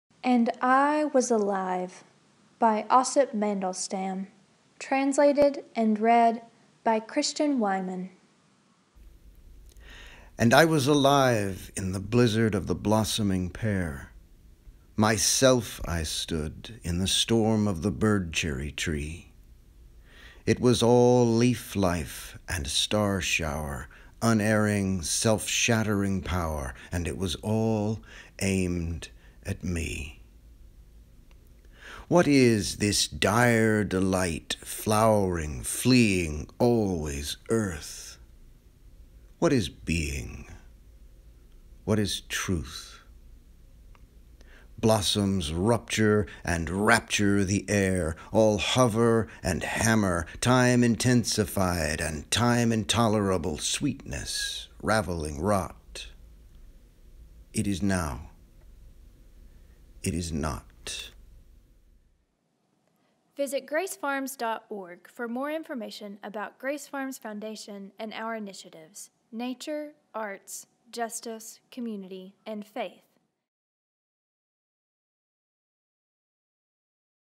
Poetry Shorts | Osip Mandelstam: And I Was Alive
"And I Was Alive"
by Osip Mandelstam
translated and read by Christian Wiman
On the occasion of our one-year anniversary, the Grace Farms Library is delighted to present a selection of poems read by their authors. The first three readings in this series feature works from Grace Farms Foundation's inaugural publication "and souls are candles," an anthology edited by Christian Wiman. Since opening in October 2015 considering texts out loud, together has become for us an important literary practice, one which we are excited to share. Turn up your volume!